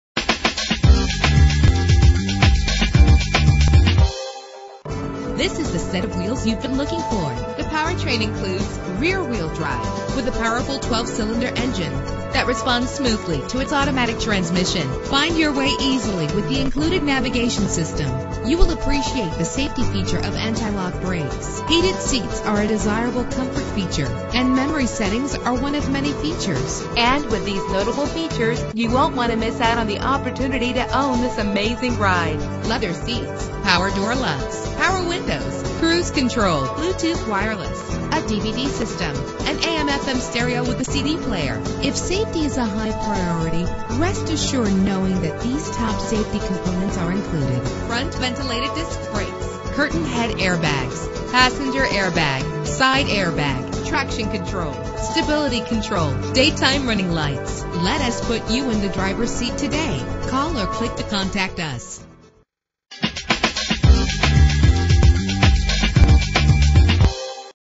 2012 Rolls-Royce Ghost - Parsippany NJ
Year: 2012
Make: Rolls-Royce
Model: Ghost
Trim: Base
Engine: 6.6 liter V12 48 valve
Transmission: 8-Speed Automatic
Color: English White
Mileage: 3661
Address:
250 Route 46 West